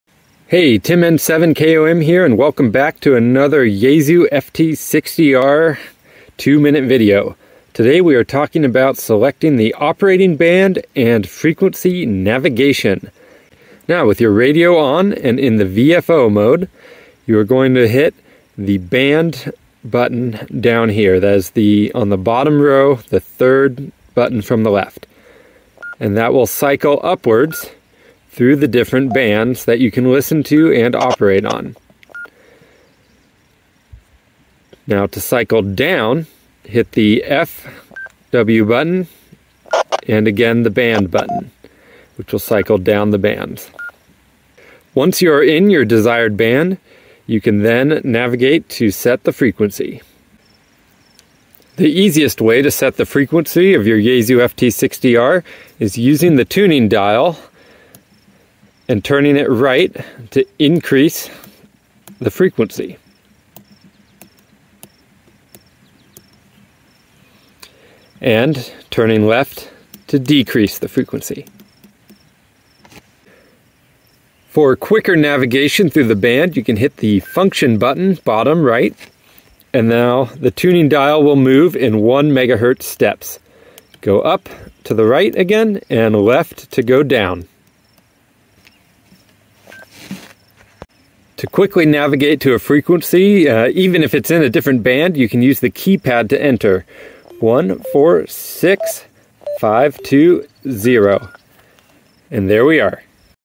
How to Change Bands and Select Frequencies | Yaesu FT-60r Series
How to change bands and select frequencies on the Yaesu FT-60r.

The Yaesu FT-60 is my favorite HT for Summits on the Air operations. In this video series, we're taking a deep dive to understand ALL aspects of this radio.